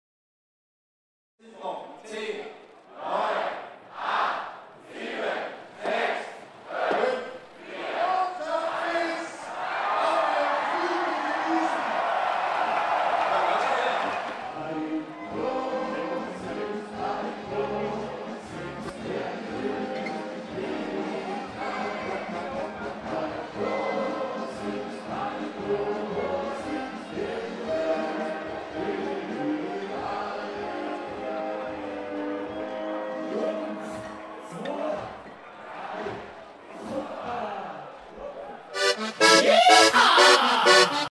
O´zapft is!
Das 180 Oktoberfest wurde vom Münchner Oberbürgermeister, Christian Ude mit zwei schlägen eröffnet. Unerstützende Worte erhielt er vom bayerischen Ministerpräsidenten Horst Seehofer im Schottenhamel auf der Wiesn.